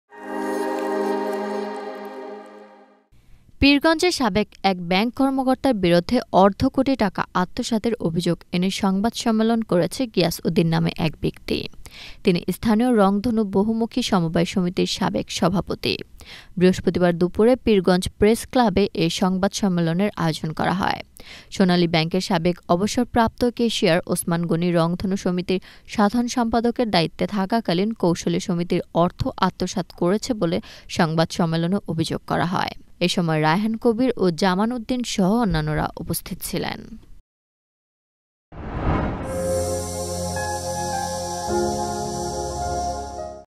পীরগঞ্জে সাবেক এক ব্যাংক কর্মকর্তার বিরুদ্ধে টাকা আত্মসাতের অভিযোগ Channel S News | Channel S 2023
পীরগঞ্জে সাবেক এক ব্যাংক কর্মকর্তার বিরুদ্ধে অর্ধকোটি টাকা আত্মসাতের অভিযোগে সংবাদ সম্মেলন অনুষ্ঠিত

1: News :
2: Entertainment News :
3: Sports News :